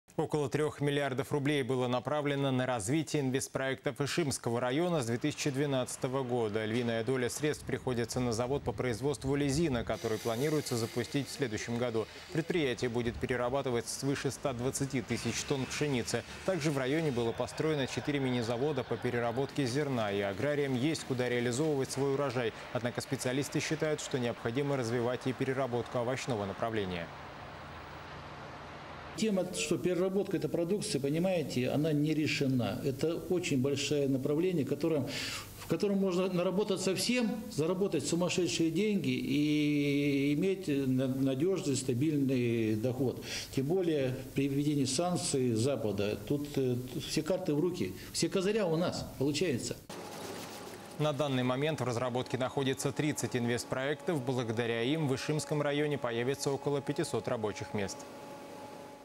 В Ишимском районе появится почти 500 рабочих мест
Около трёх миллиардов рублей было направлено на развитие инвестпроектов Ишимского района с 2012 года. Львиная доля средств приходится на завод по производству лизина, который планируется запустить в следующем году. Предприятие будет перерабатывать свыше 120 тысяч тонн пшеницы. Также в районе было построено 4 мини-завода по переработке зерна. И аграриям есть куда сбывать свой урожай. Однако, специалисты считают, что необходимо развивать и переработку овощного направления.
На данный момент в разработке находится 30 инвестпроектов.
 Благодаря им, в Ишимском районе появится около 500 рабочих мест.